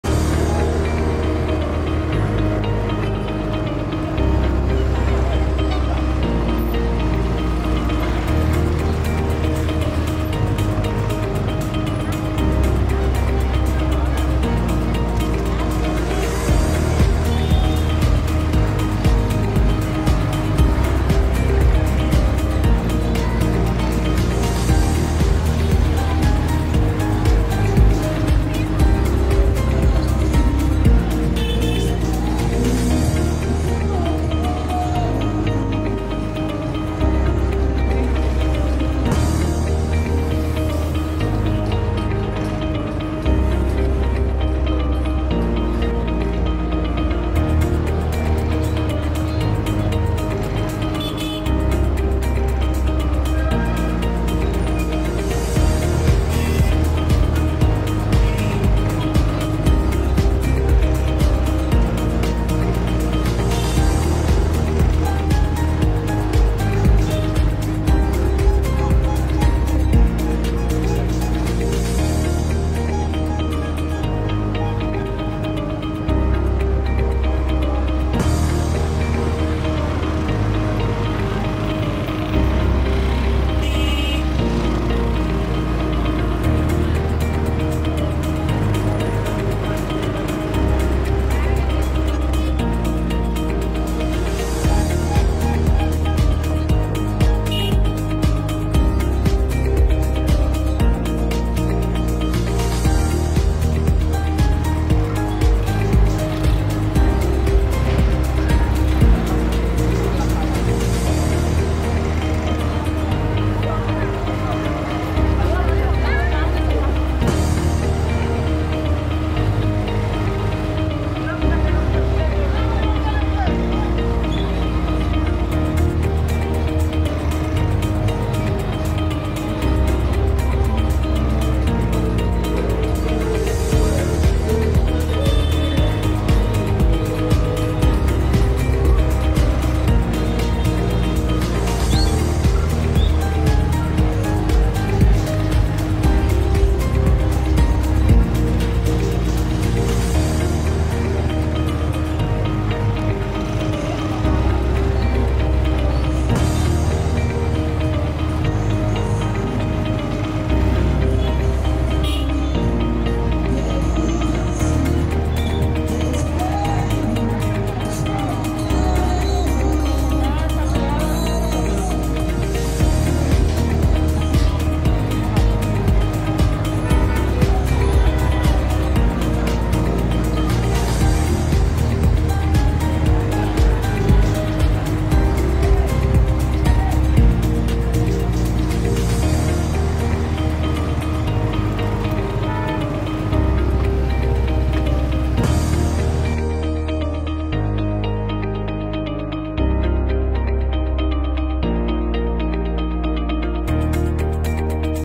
SEATOU FALHA ORGANIZA NEGOSIANTE OKUPA ESPASU PUBLIKU IHA DILI IHA TEMPU KALAN
SEATOU FALHA ORGANIZA NEGOSIANTE OKUPA ESPASU  PUBLIKU
IHA  DILI IHA TEMPU KALAN IHA KAMPUN BARU DILI